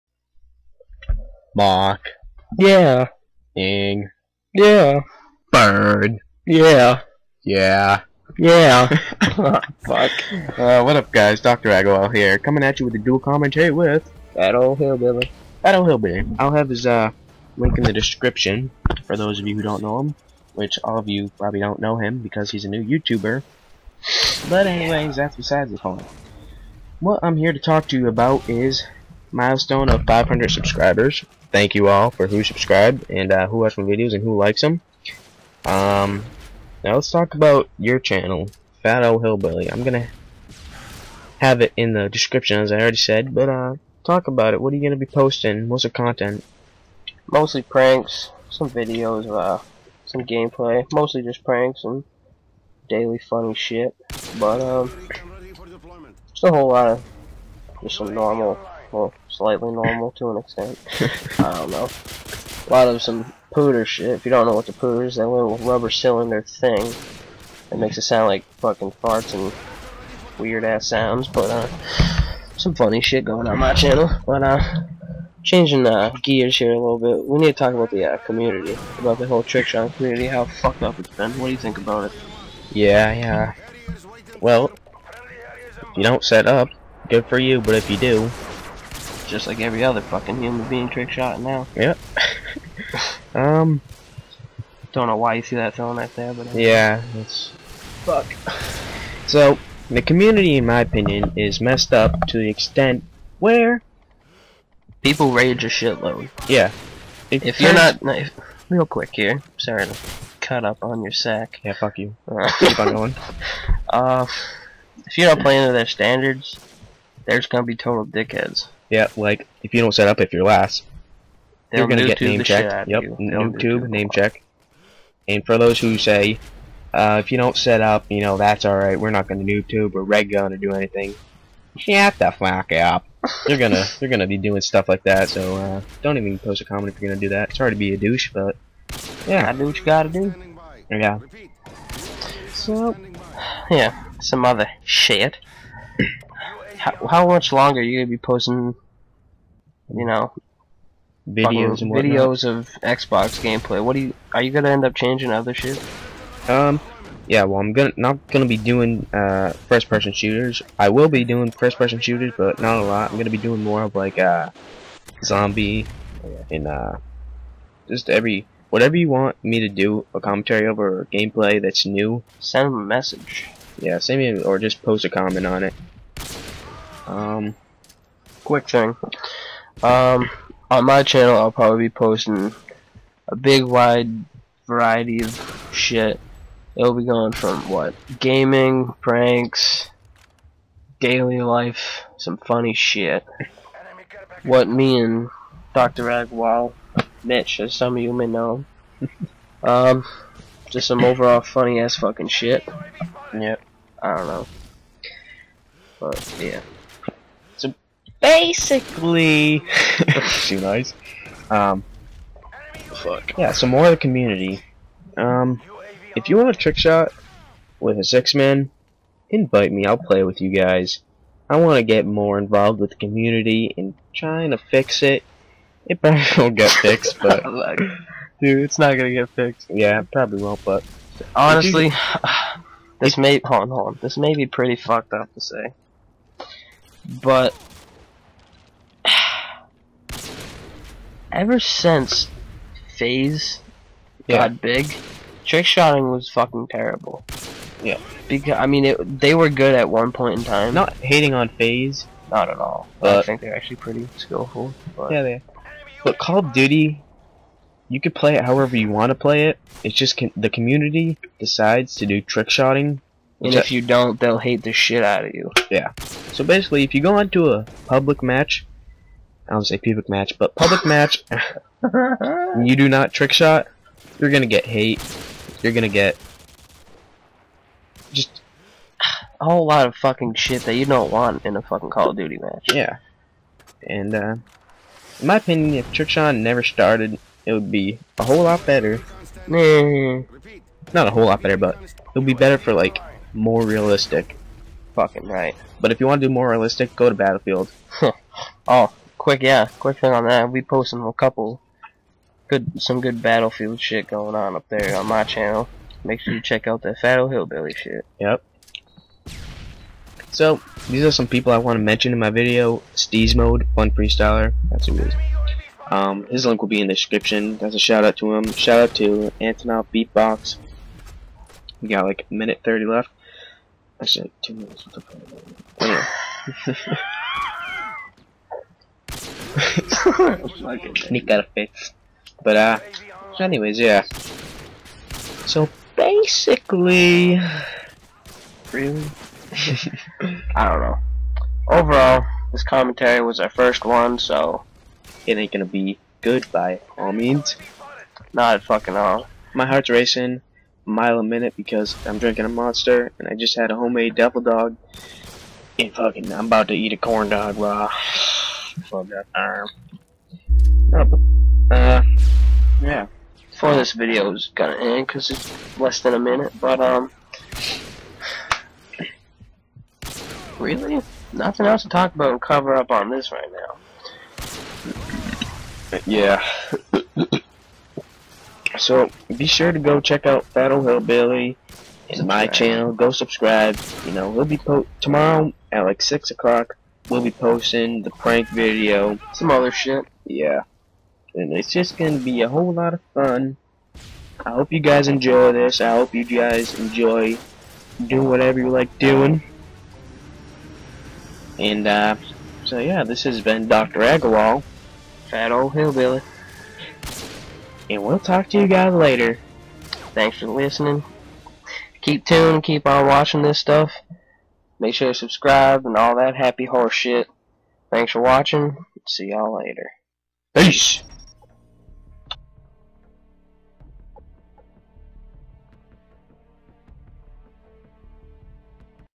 Dual Commentary
Anyways this commentary talks about what i will be doing in the future as well as ryan. Thank Yoooouuuuu!